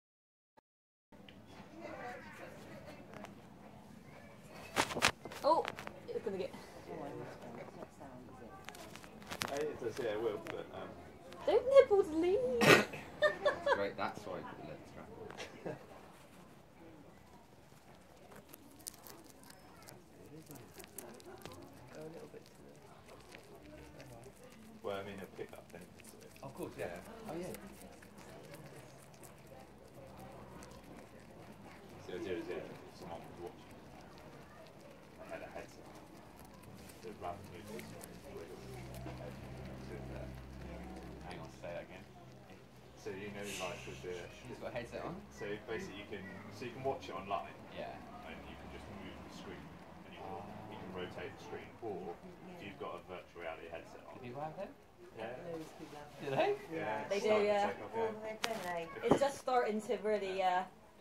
Rabbits filmed in 360° at Windmill Hill City Farm
Try watching with a VR headset.